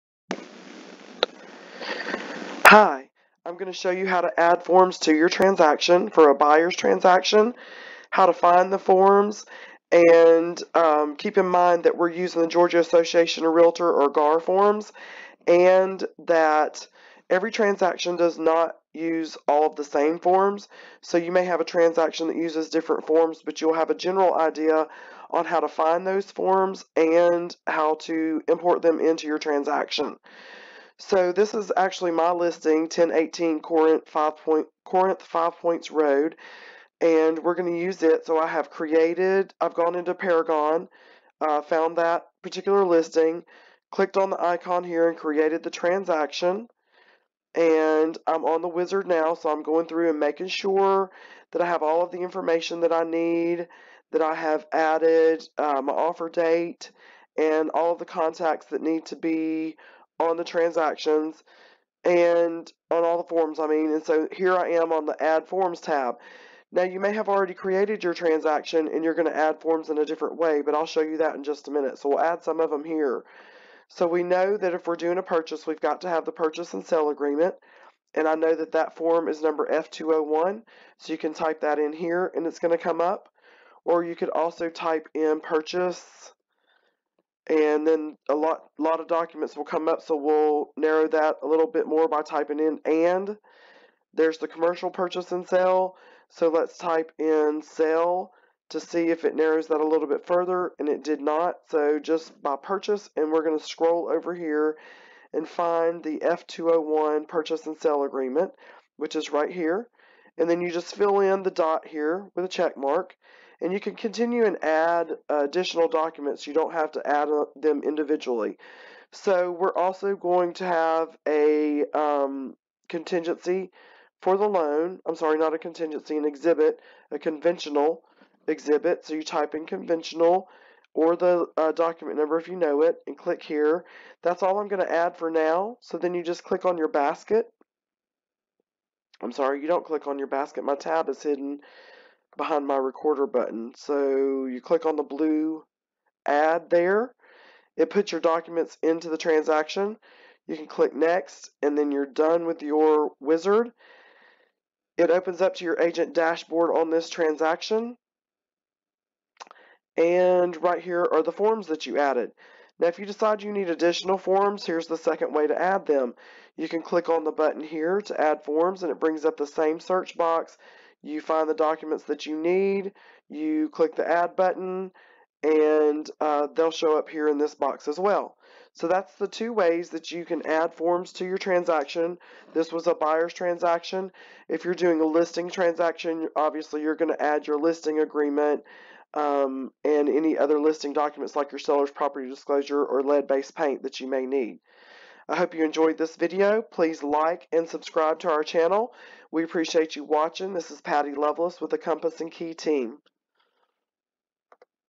How to Add Forms to A Transaction Desk Transaction
Adding forms for the transaction may be done individually or in bulk.  This quick video walks you through how to do either with ease.  If you have any questions, get our email towards the end of the video.  We would love to hear from you.